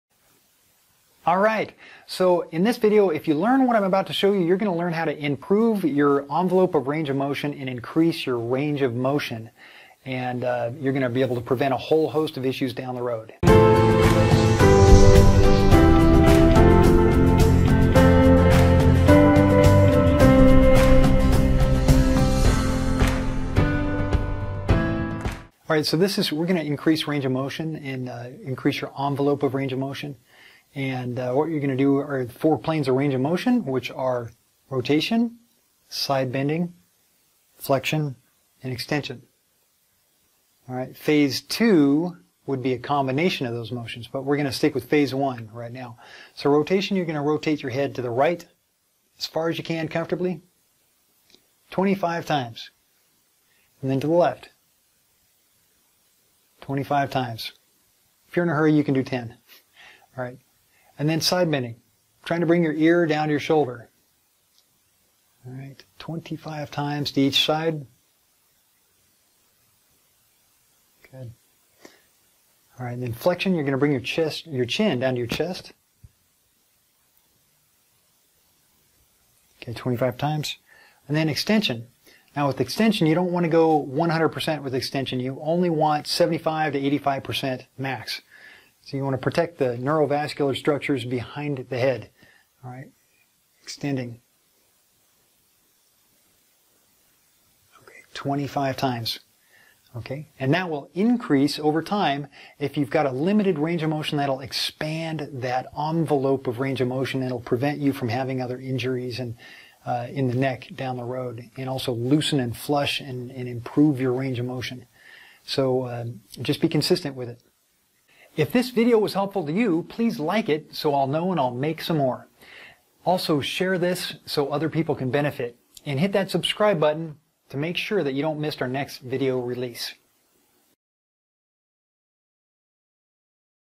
Neck Pain Relief - Increasing Range of Motion
Looking for neck pain relief? One of the best ways to get neck pain relief is by lengthening tight short muscles that are holding tension in the neck. This video will show you an easy range of motion movements that will allow you to get your range of motion back, and keep it! In addition to increasing your envelope of range of motion which will dramatically reduce your chances of neck injury.

Go slow and easy on your neck at first, or you might be sore. Don't overwork yourself or be too aggressive, especially if you haven't had much work done on your neck. Try to use a combination of some of my other self-neck massage techniques for more relief.

Sometimes getting out of pain and feeling better is not a "straight line" (from A to B), sometimes it's "U" shaped, and we feel more uncomfortable before we start to feel better.

Regardless, If you start to have significantly more discomfort for whatever reason, get it checked out by a qualified professional.